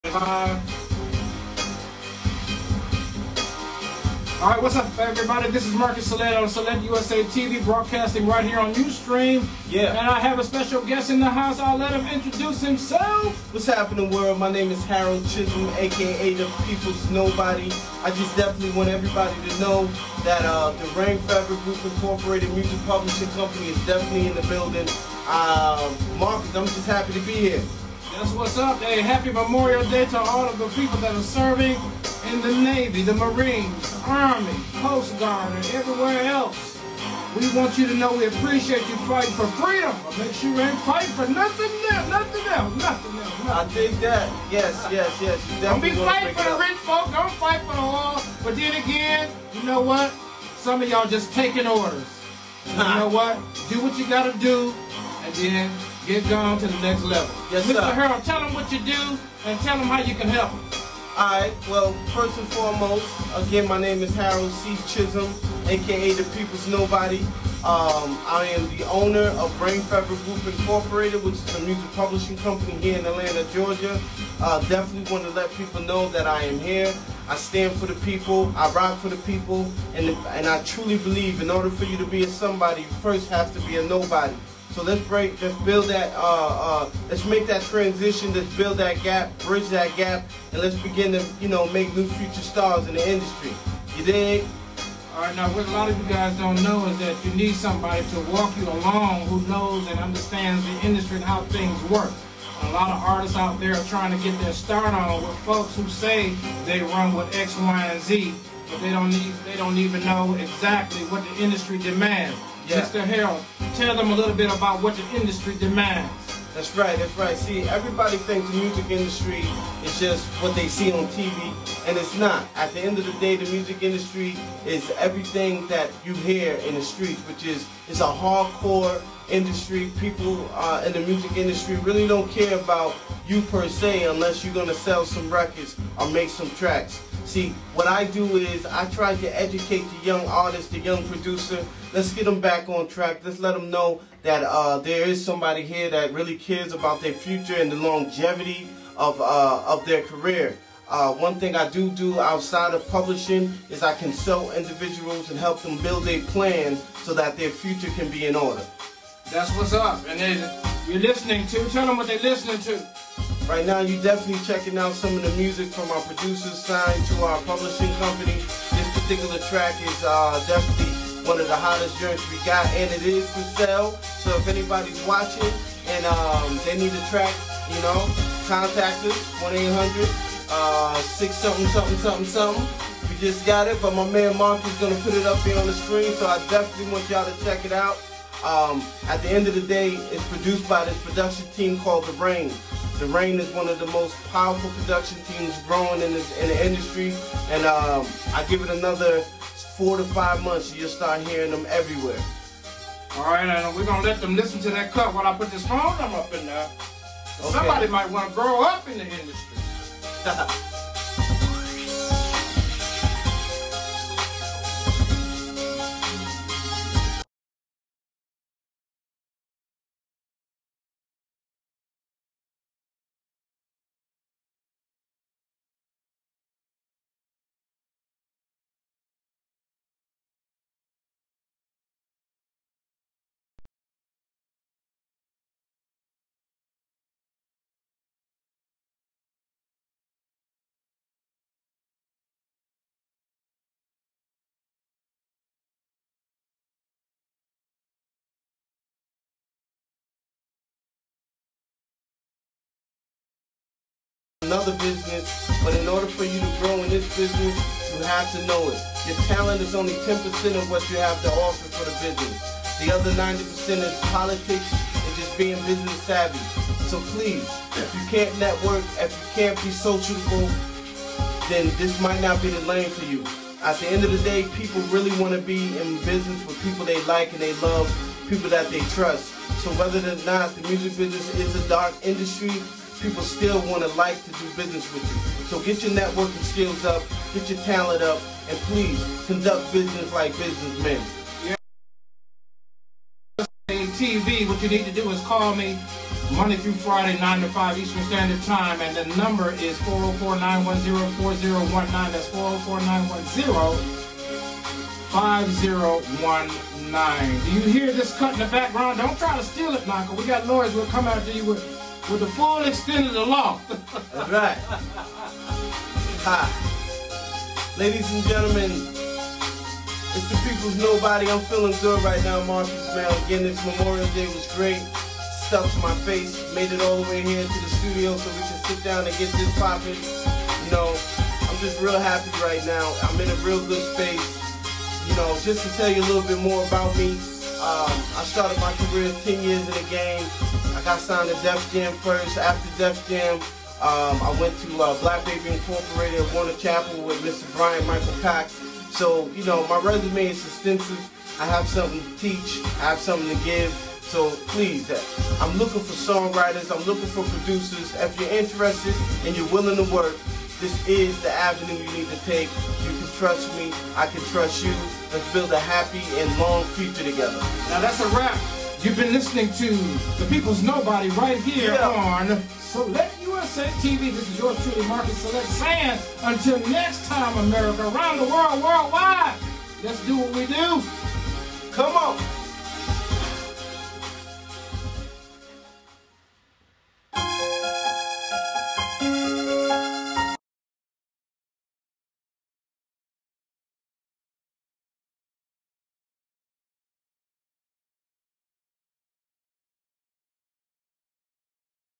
The Peoples Nobody...
The Peoples Nobody shares how to get involved in the music business... Get discovered right NOW and put yourself on the map...GROW UP in the industry right... - Captured Live on Ustream